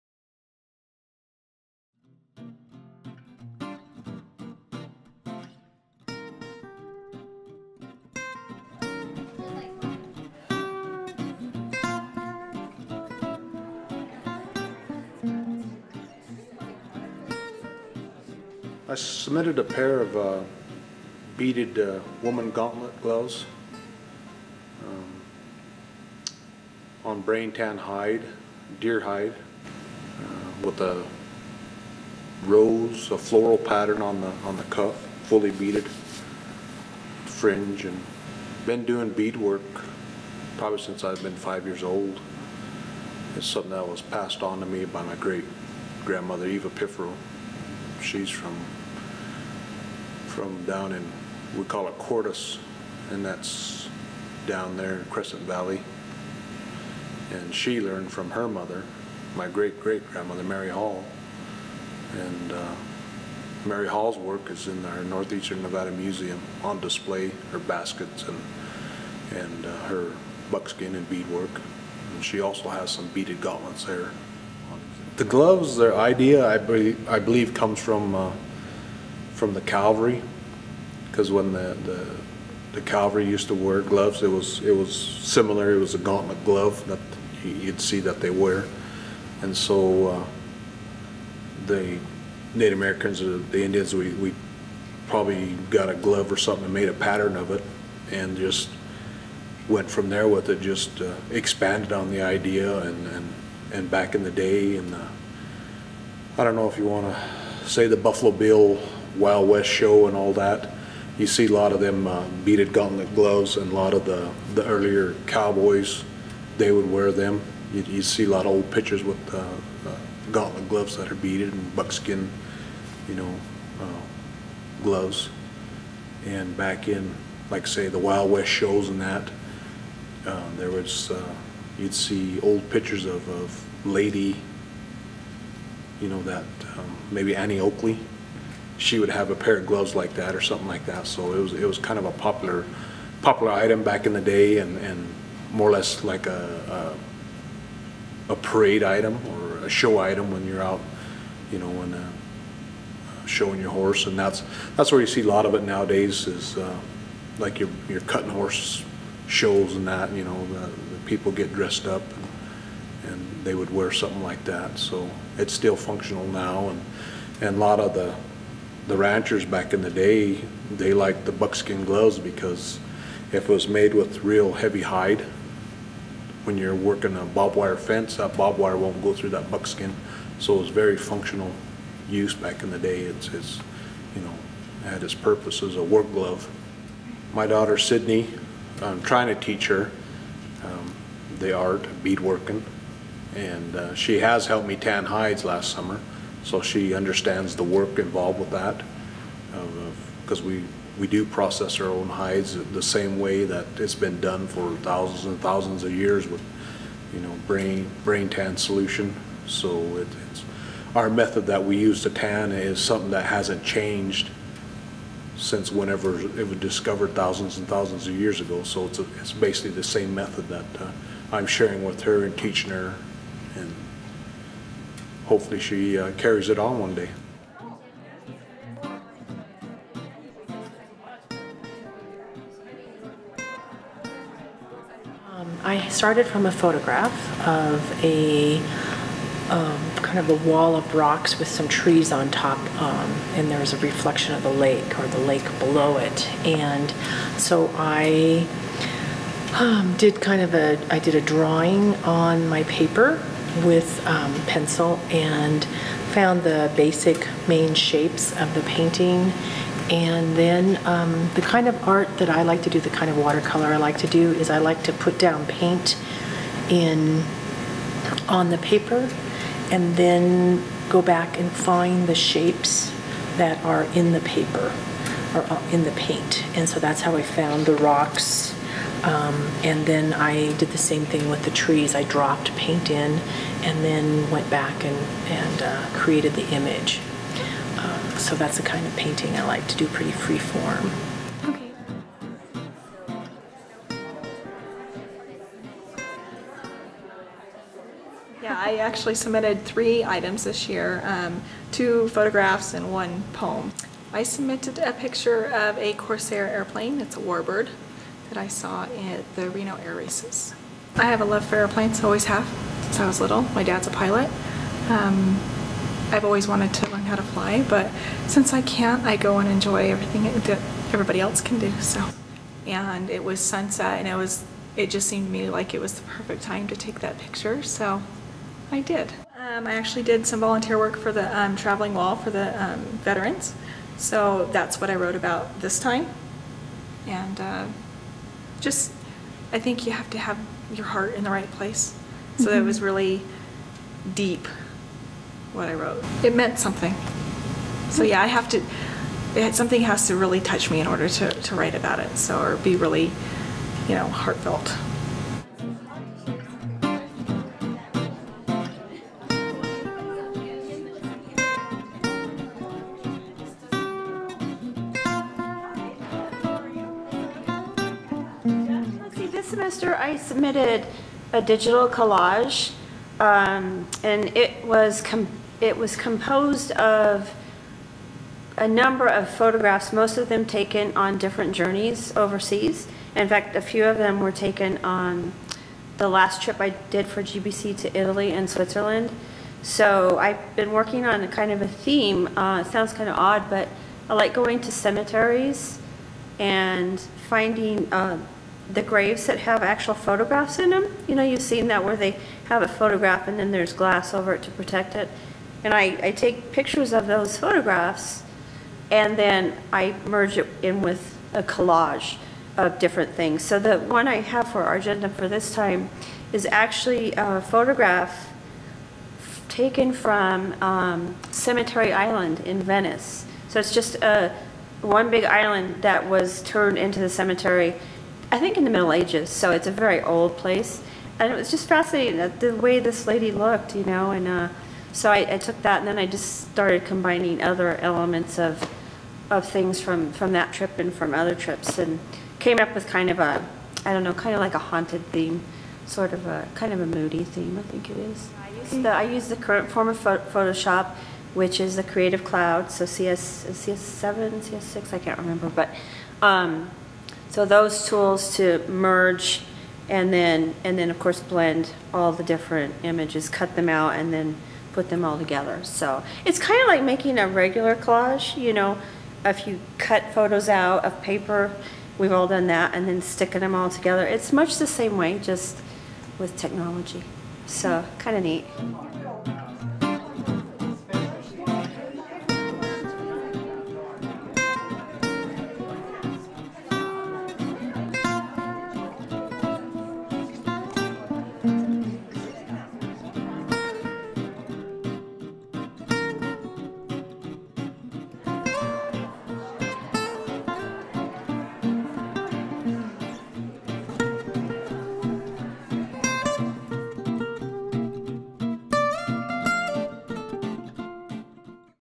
Argentum and Student Art Show Spring 2015
Argentum, Great Basin College's art and literary magazine, is devoted to highlighting the excellent artistic expression of its students, faculty and staff. Community members are also encouraged to submit work, but must be at least 18 years old. This video highlights our Spring 2015 Art Exhibit, featuring artists Shawn Collins, Debbie Heaton-Lamp, Kacie Ortiz and Cynthia Delaney.Cited Historical photos' links: allposters.com, icollector.com, and blog4history.com